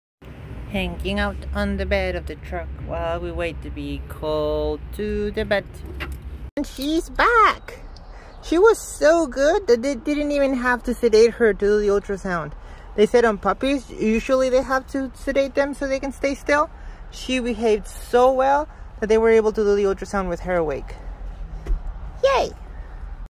Taking Luna for her Ultrasound
Had to take Luna for her Ultrasound